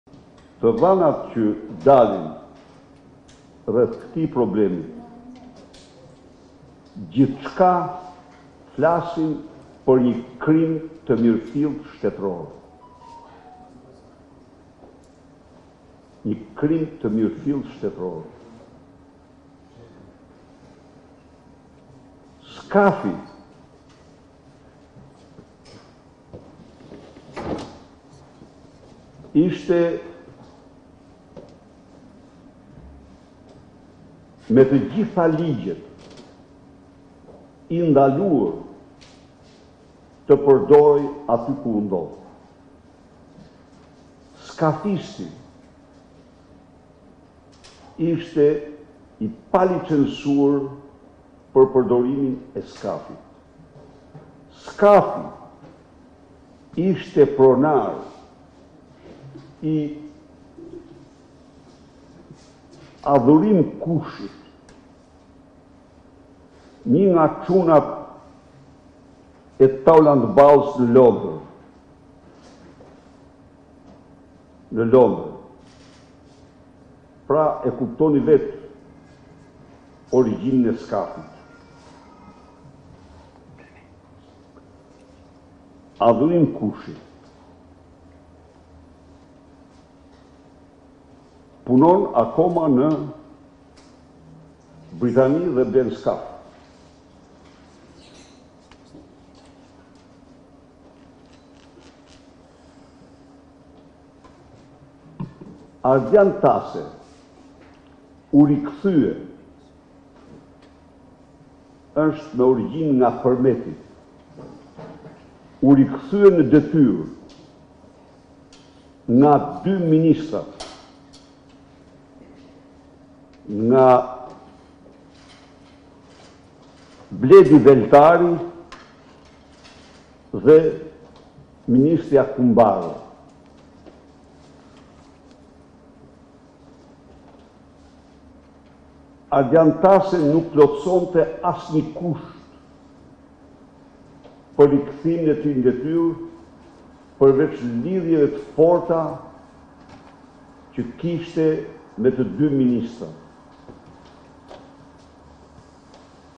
Report TV -Berisha: Pronari i skafit ishte Adhurim Kushi, punon në Britani!
Sali Berisha ka reaguar sërish për ngjarjen e rëndë në Himarë, ku 7-vjeçarja Jonada Avdia u vra nga gomonia e drejtuar nga komisari Arjan Tase. Në konferencën për mediat, Berisha tha se pronari i skafit ishte Adhurim Kushi që punon në Britani dhe atje bën blerjet e skafeve. Ndërsa për Arjan Tasen thotë se nuk plotësonte kushtet për rikthimin në detyrë, por kishte lidhje të forta me ministrin Bledi Cuci dhe ministren Mirela Kumbaro.